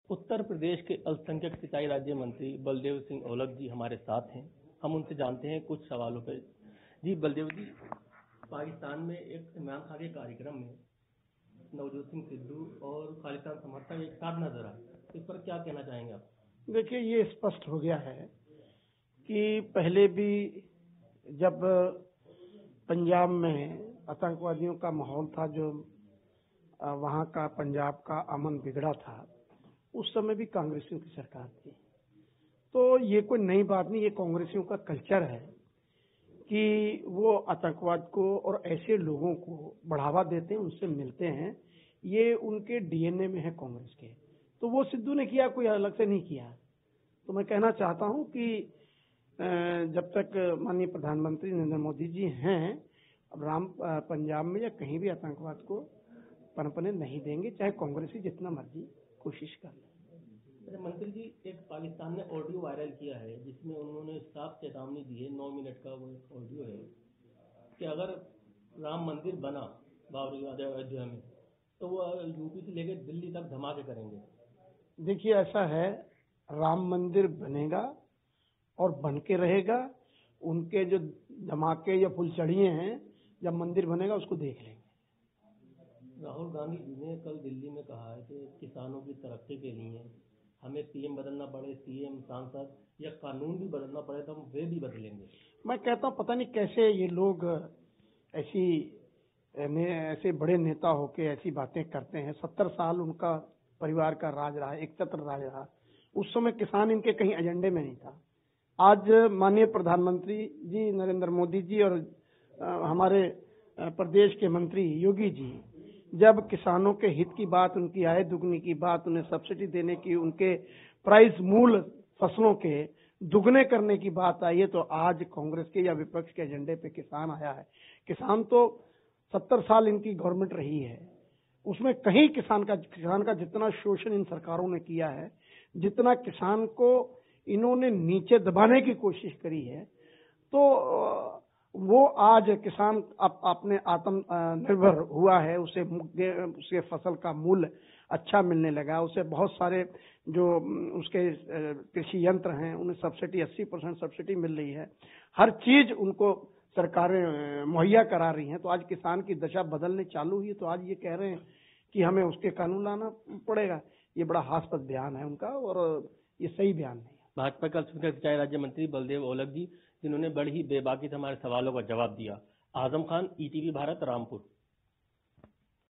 आतंकवाद को बढ़ावा देना कांग्रेस के डीएनए में है : सिंचाई राज्य मंत्री बलदेव सिंह
रामपुर : यूपी  के अल्पसंख्यक सिंचाई राज्य मंत्री बलदेव सिंह से उनके आवास पर हमने कुछ खास मुद्दों पर बातचीत की राज्यमंत्री ने कांग्रेस पार्टी पर निशाना साधाते हुए उन्होंने कहा कि पंजाब में जब अमन बिगड़ा था उस समय भी कांग्रेस की सरकार थी. इनका कल्चर है आतंकवाद को बढ़ावा देना कांग्रेस के डीएनए में ही यह बसा हुआ है.